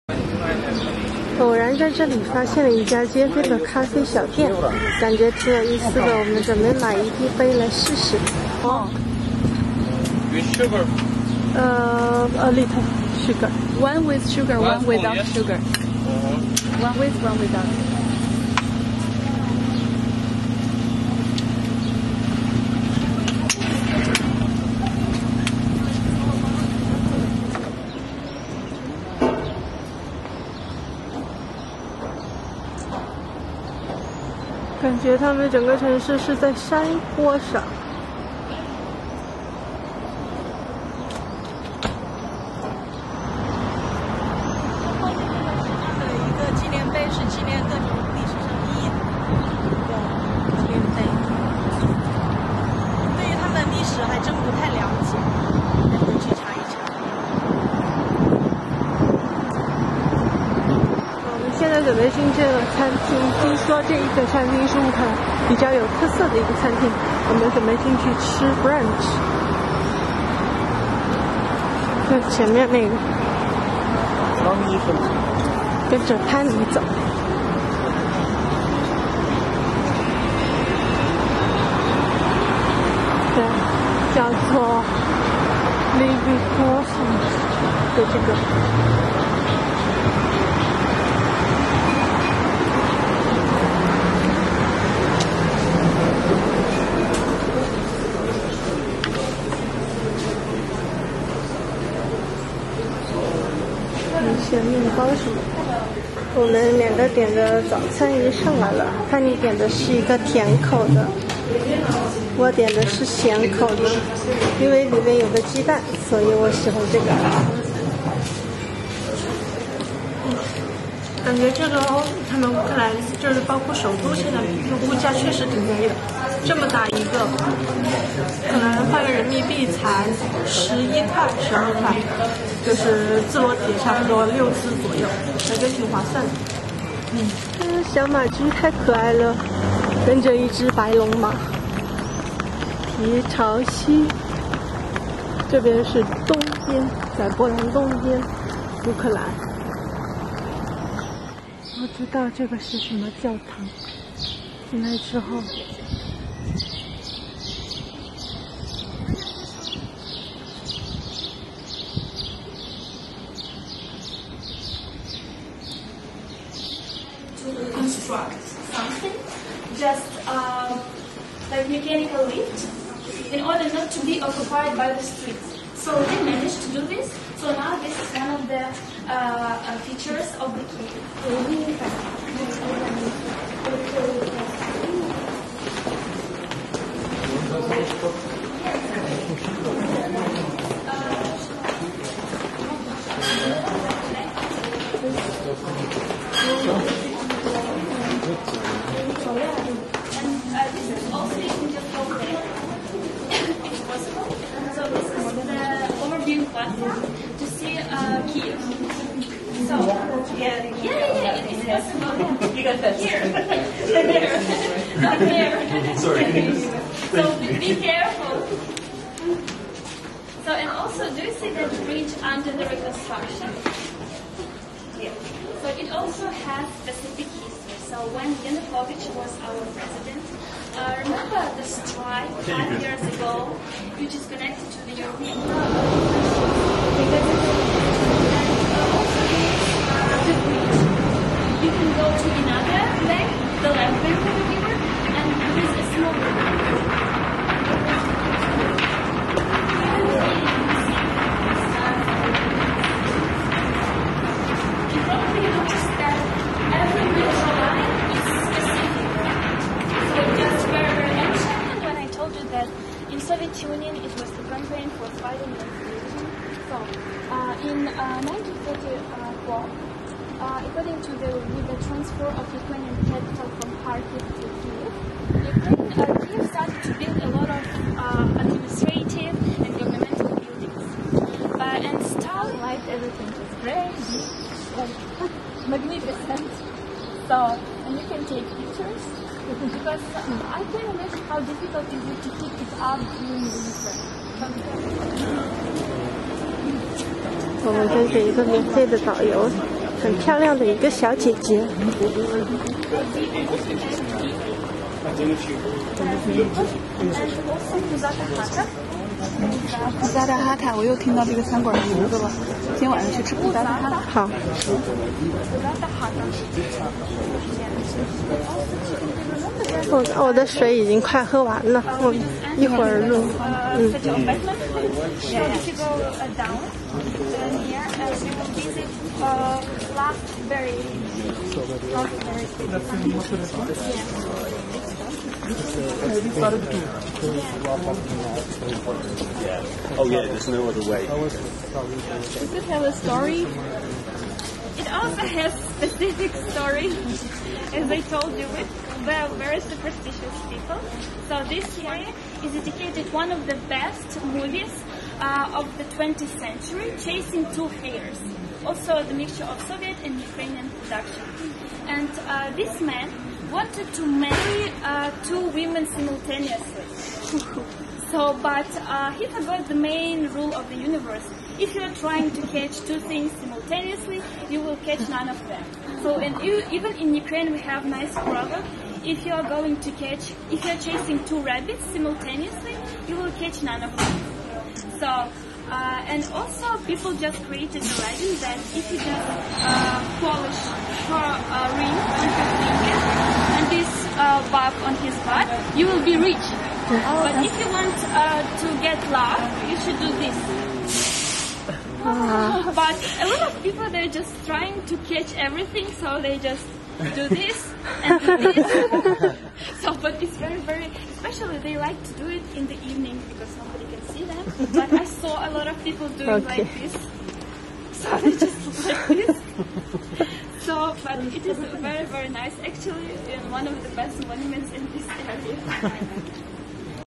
乌克兰旅行之基辅篇（二）Kiev HD 1080p
6月20日，同一天，半路碰到了一个导游小姐姐，所以我们就跟着她走了，因为跟着听她的介绍更详细😊。